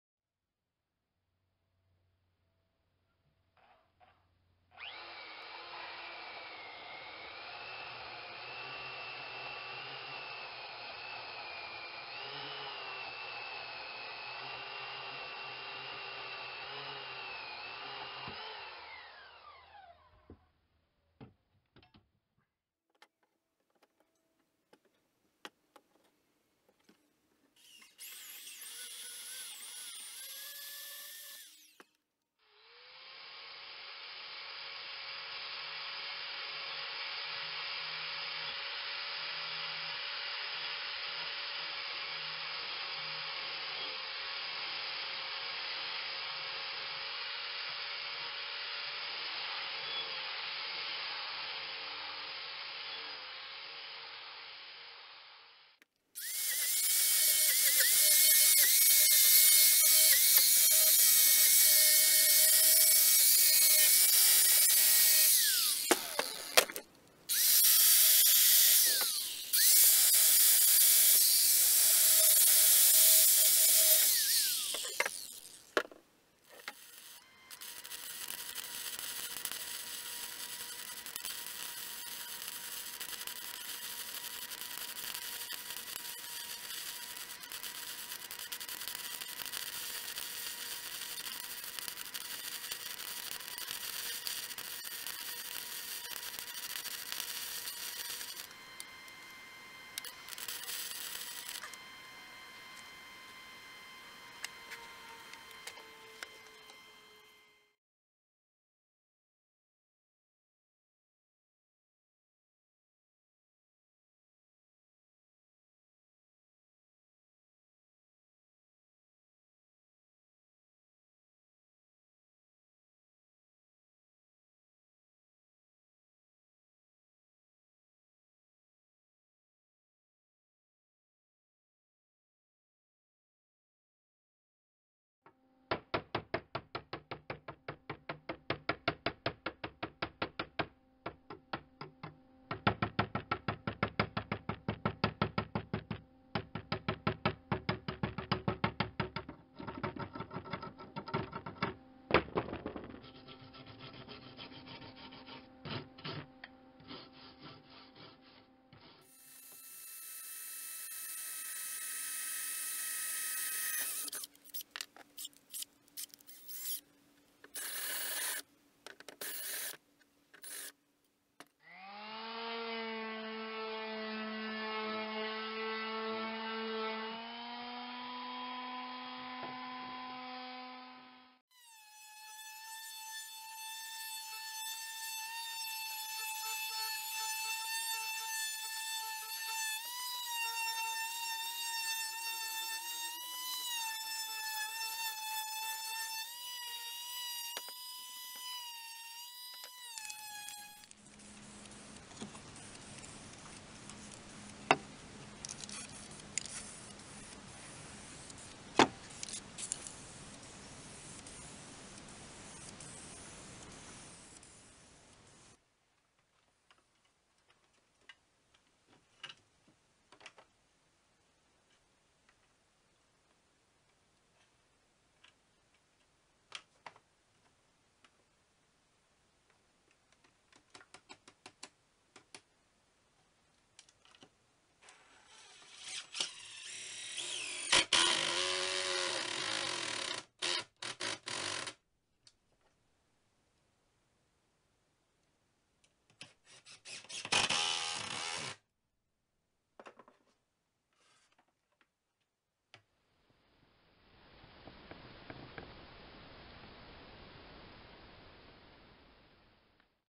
Magnetic Sewing Pin Holder
A quick and easy metal bar attached to a piece of wood mounted on the wall gives a handy place for sewing pins that are in a magnetic tray. We'll be adding one more at the pinning station as well.